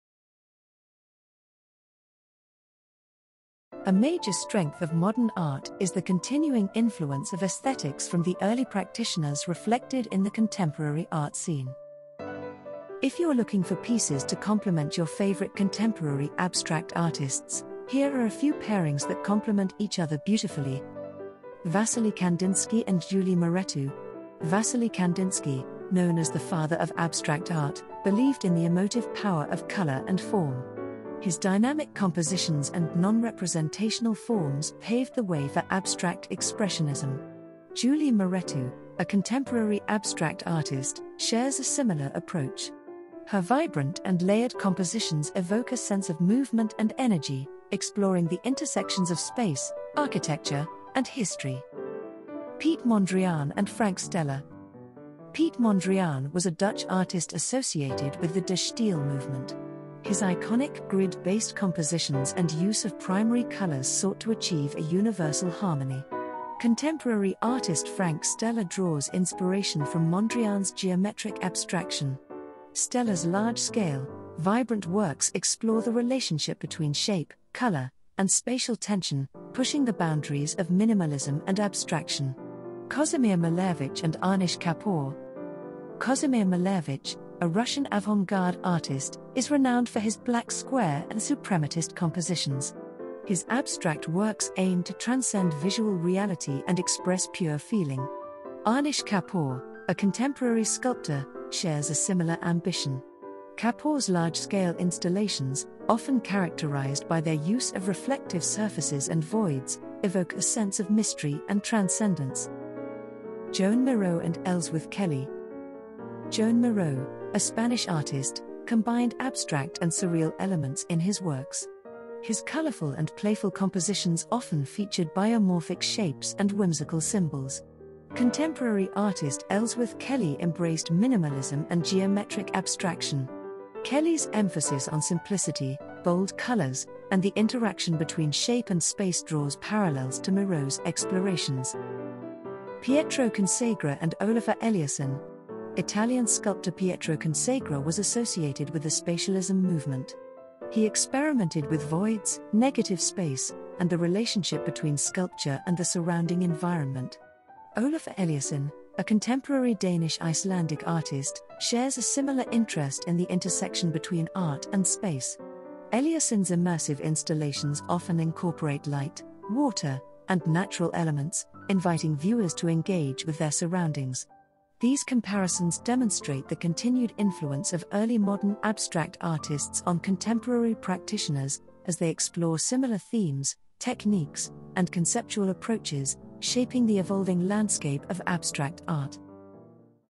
Then & Now: Early Modern and Contemporary artists - 10 Artists whose artworks match beautifully.
A major strength of modern art is the continuing influence of aesthetics from the early practitioners reflected in the contemporary art scene.

If you’re looking for pieces to compliment your favorite contemporary abstract artists, here are a few pairings that compliment each other beautifully:

Wassily Kandinsky and Julie Mehretu:
Wassily Kandinsky, known as the father of abstract art, believed in the emotive power of color and form. His dynamic compositions and non-representational forms paved the way for abstract expressionism. Julie Mehretu, a contemporary abstract artist, shares a similar approach. Her vibrant and layered compositions evoke a sense of movement and energy, exploring the intersections of space, architecture, and history.

Piet Mondrian and Frank Stella:
Piet Mondrian was a Dutch artist associated with the De Stijl movement. His iconic grid-based compositions and use of primary colors sought to achieve a universal harmony. Contemporary artist Frank Stella draws inspiration from Mondrian's geometric abstraction. Stella's large-scale, vibrant works explore the relationship between shape, color, and spatial tension, pushing the boundaries of minimalism and abstraction.

Kazimir Malevich and Anish Kapoor:
Kazimir Malevich, a Russian avant-garde artist, is renowned for his Black Square and Suprematist compositions. His abstract works aimed to transcend visual reality and express pure feeling. Anish Kapoor, a contemporary sculptor, shares a similar ambition. Kapoor's large-scale installations, often characterized by their use of reflective surfaces and voids, evoke a sense of mystery and transcendence.

Joan Miró and Ellsworth Kelly:
Joan Miró, a Spanish artist, combined abstract and surreal elements in his works. His colorful and playful compositions often featured biomorphic shapes and whimsical symbols. Contemporary artist Ellsworth Kelly embraced minimalism and geometric abstraction. Kelly's emphasis on simplicity, bold colors, and the interaction between shape and space draws parallels to Miró's explorations.

Pietro Consagra and Olafur Eliasson:
Italian sculptor Pietro Consagra was associated with the Spatialism movement. He experimented with voids, negative space, and the relationship between sculpture and the surrounding environment. Olafur Eliasson, a contemporary Danish-Icelandic artist, shares a similar interest in the intersection between art and space. Eliasson's immersive installations often incorporate light, water, and natural elements, inviting viewers to engage with their surroundings.

These comparisons demonstrate the continued influence of early modern abstract artists on contemporary practitioners, as they explore similar themes, techniques, and conceptual approaches, shaping the evolving landscape of abstract art.

Image credits: Julie Mehretu, Tom Powel Imaging, Whitney Museum of American Art, observer.com, howtoacademy.com, thecollector.com, Runa Maya Mørk Huber / Studio Olafur Eliasson, Fundacio Joan Miro, architecturaldigest.com, themodern.org, The Dallas Morning News, mucciaccia.com, 4columns.org, Culture Trip, USA Art News